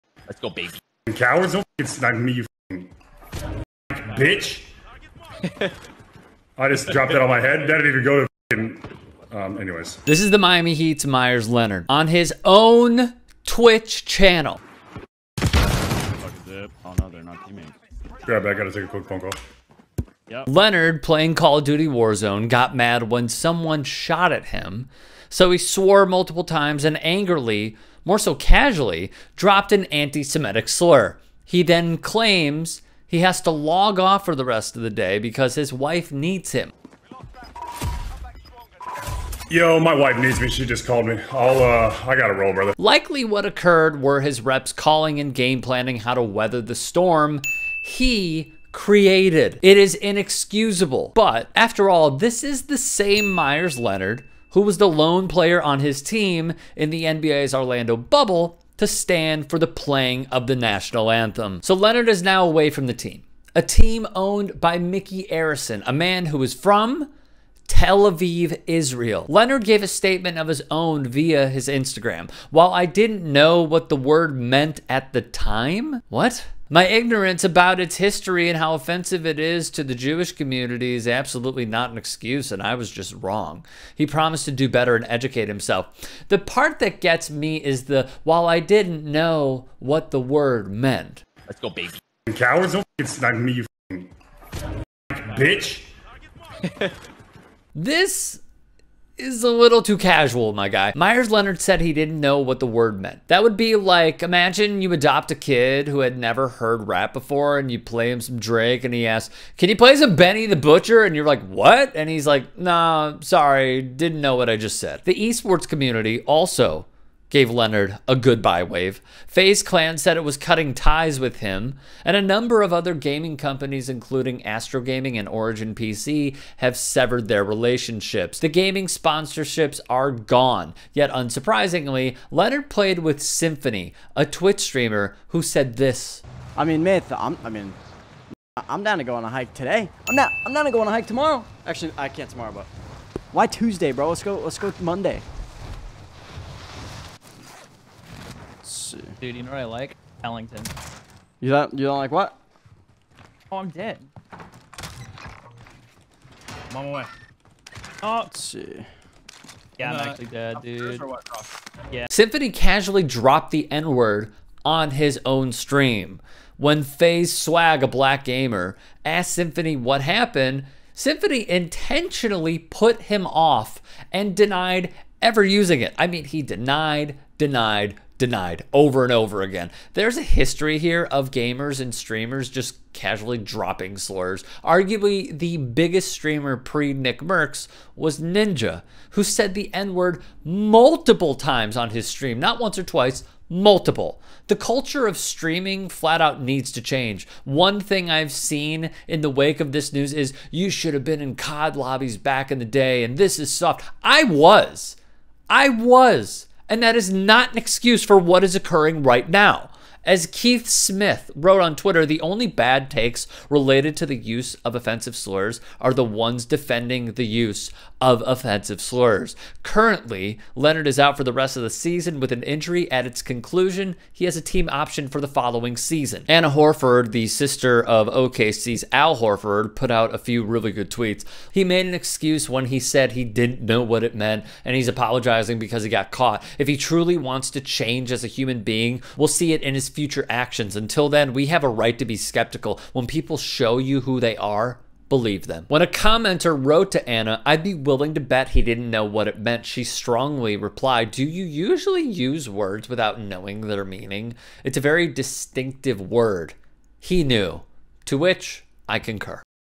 Meyers Leonard Uses Anti-Semitic Slur During Livestream
Meyers Leonard is suspended from the Heat indefinitely. Rick Strom breaks it down.

Rick Strom